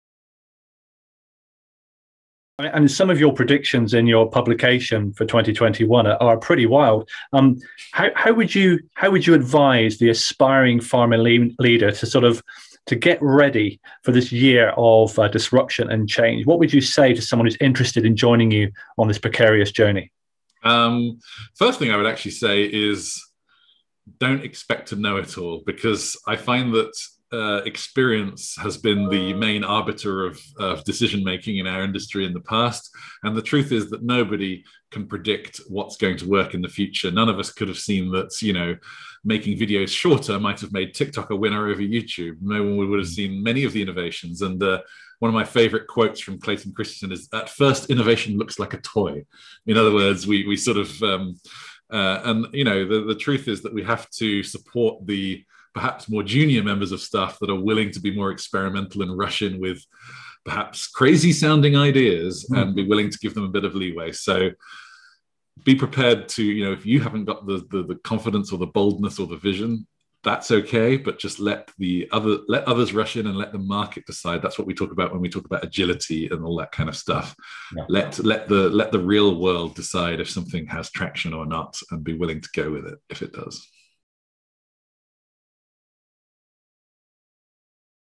How to be an innovative leader: A FiveAndCo. Interview with Paul Simms
FiveAndCo. talk to "Pharma Provocateur" Paul Simms about how to practically be an innovative leader.